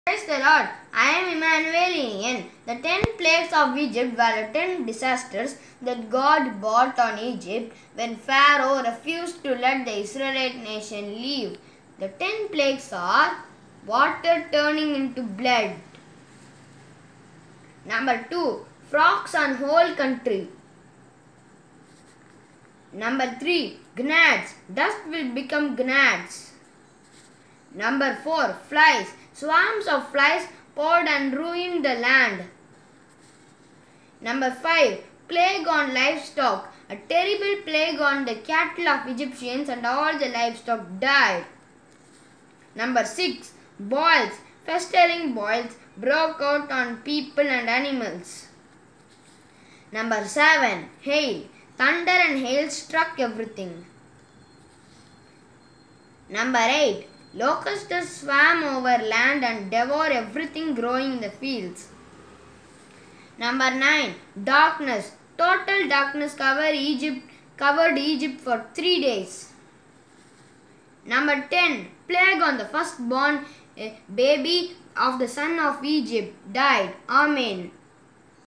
Ten Plagues upon Egypt | Miracles in Bible | Paper Spin Wheel craft | Immanuel Iniyan
Moses, a great prophet was instrumental in performing  God's deeds to free the Israelites from the slavery of the Egyptians. The ten miracles performed by Moses and Aaron with God's leading in front of the Pharoh  was considered to be the ten plagues of Egypt.

Immanuel explains the ten plagues through the paper spin wheel craft.

Stay blessed.